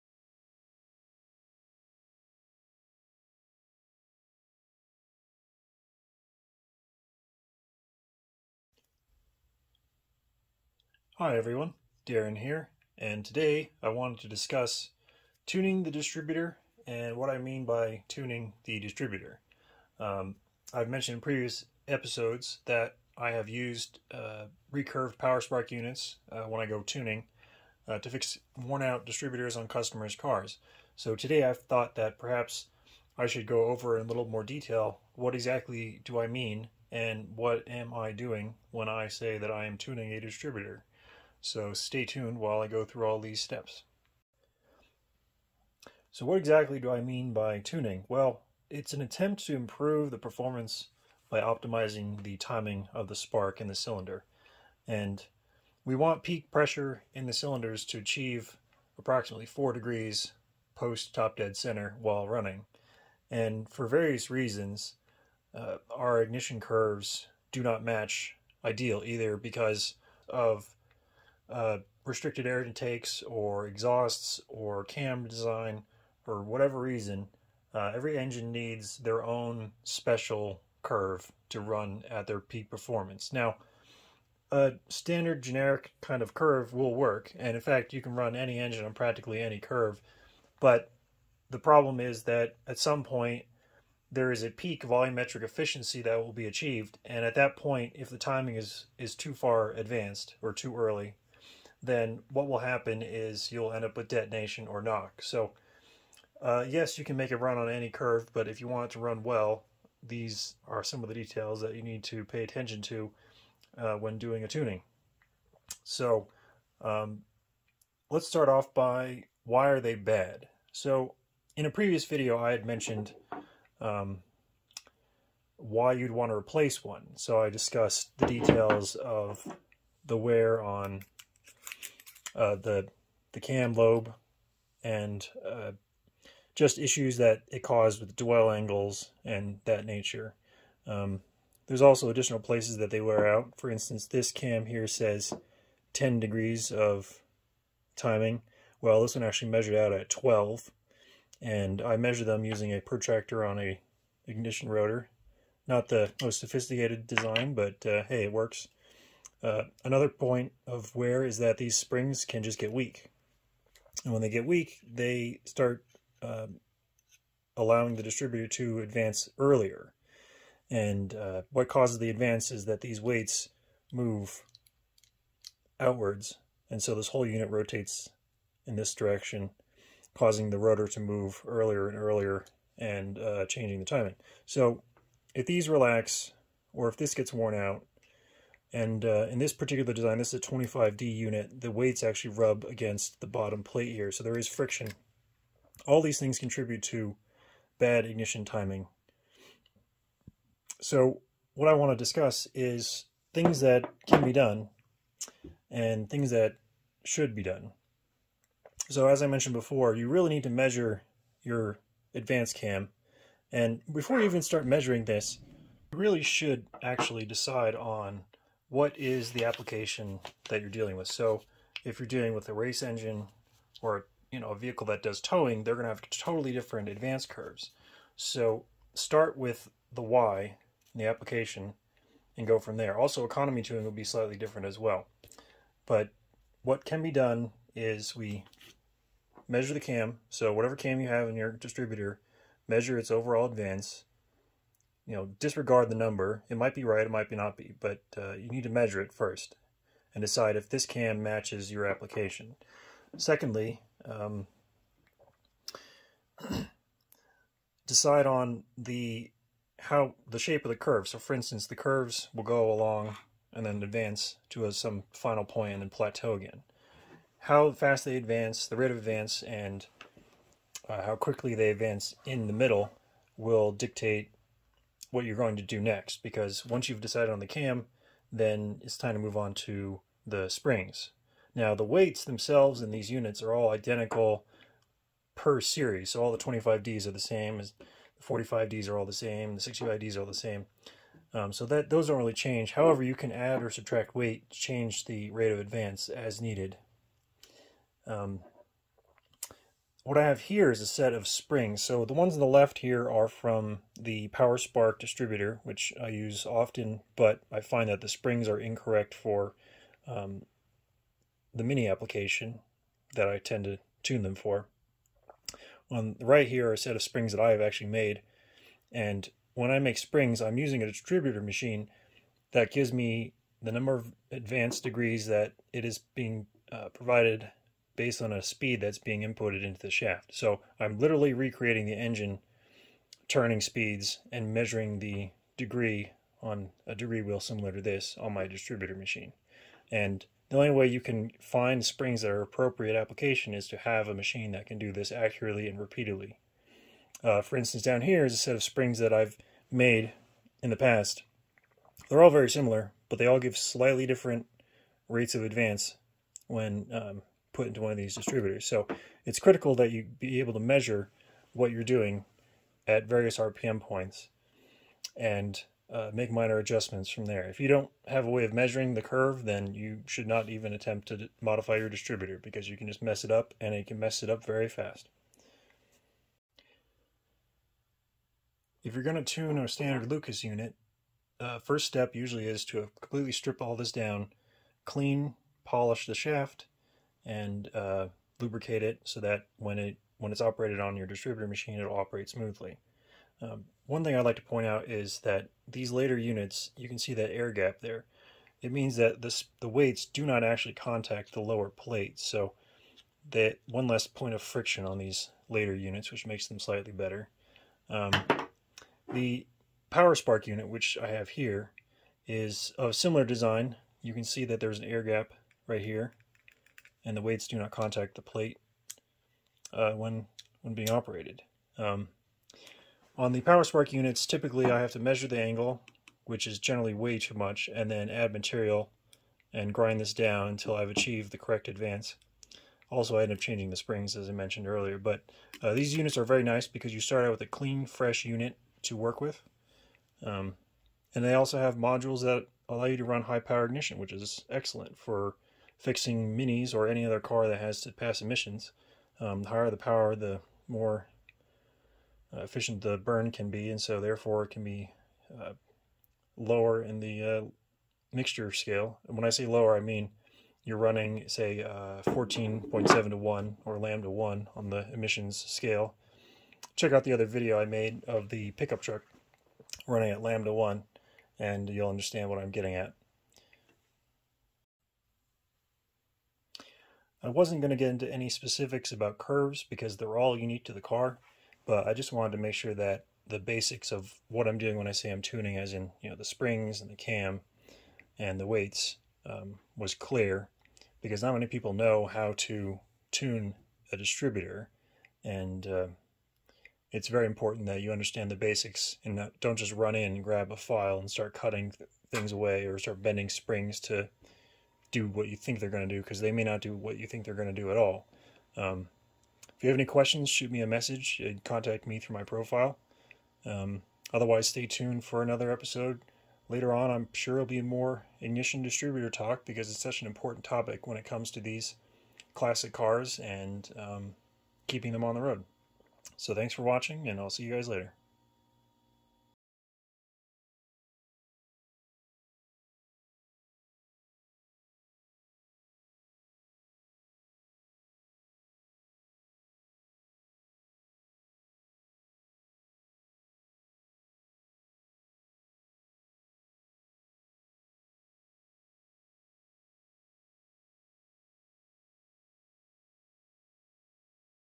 The basics of Tuning a Lucas Distributor
I discuss the basics of tuning Lucas distributors.
I go over the basics of springs, both primary and secondary.
The weights and the different types that are found in the Lucas 23, 25, 45, 65 dizzys,
The importance of measuring the advance cam and checking for wear.
The importance of checking the springs and their strength vs old worn ones.
No details on actual curves are provided, just what goes into tuning one.